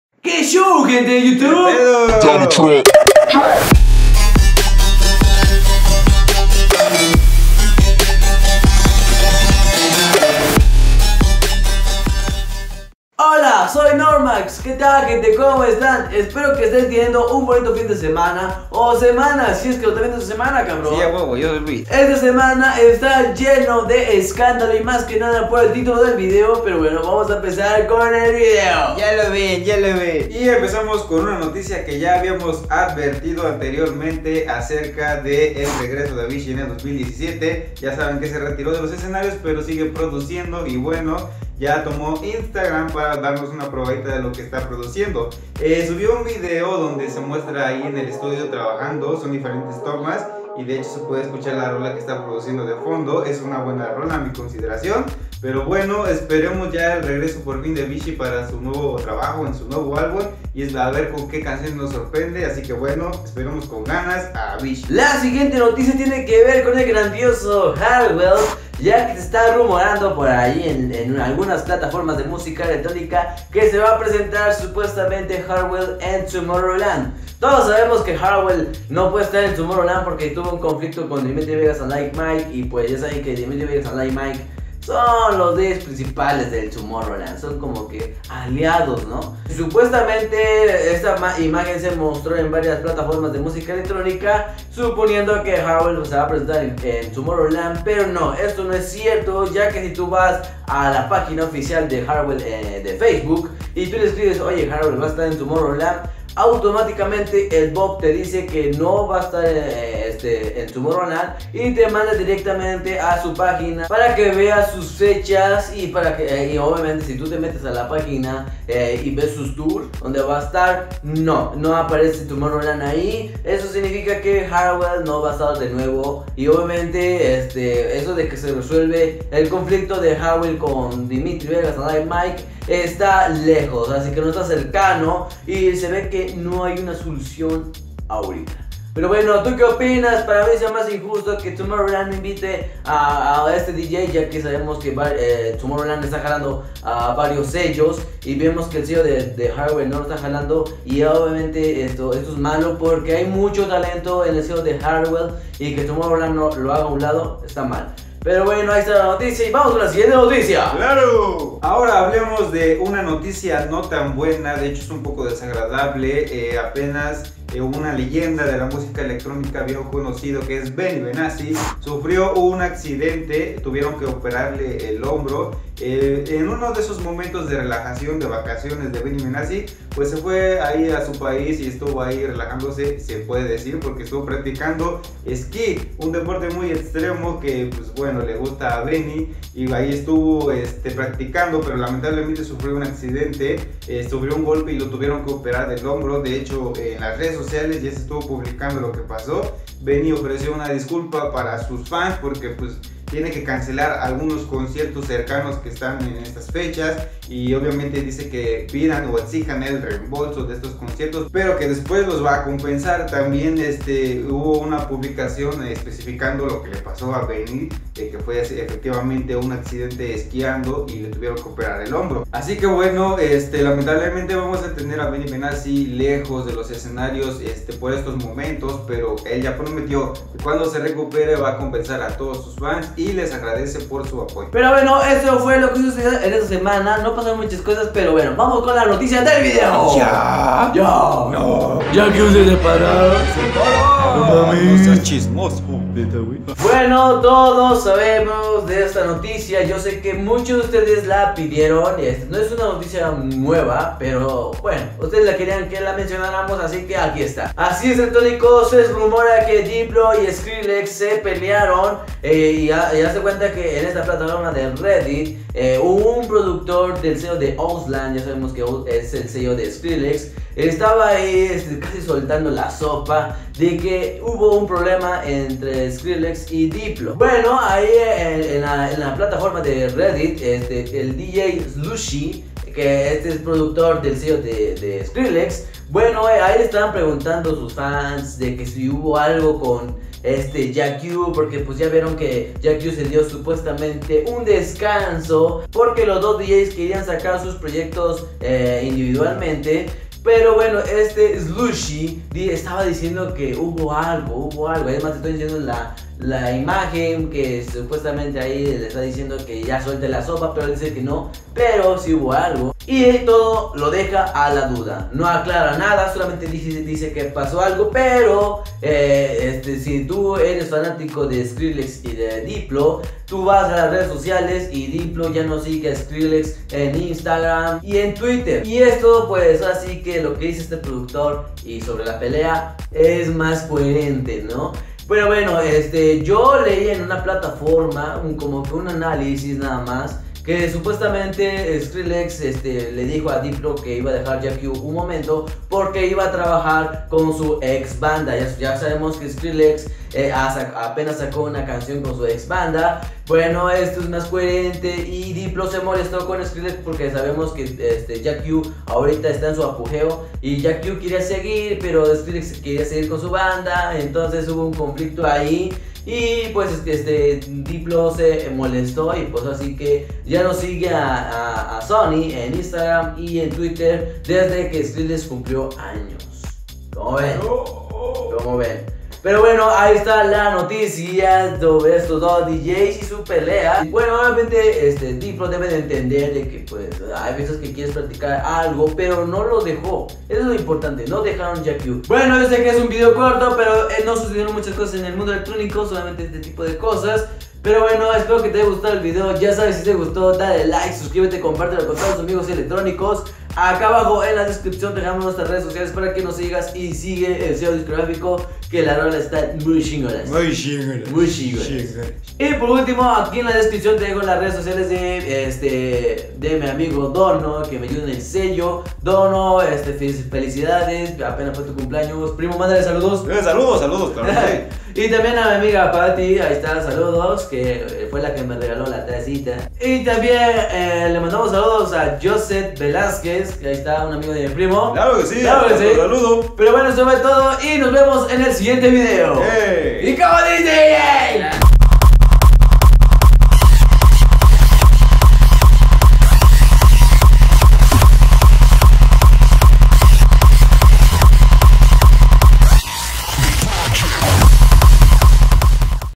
¿Diplo Se Pelea Con Skrillex? /Noticias #114
HOLA ELECTRÓNICOS! EN ESTE VÍDEO LES EXPLICO UN POCO DEL SUPUESTO CONFLICTO DE LOS MIEMBROS DE JACK U, AUN QUE RECIENTEMENTE DIPLO ACLARO QUE SE ENFOCARA EN MAJOR LAZER YA QUE SKRILLEX ESTA TRABAJANDO CON SU EX BANDA! ESPERO LES GUSTE!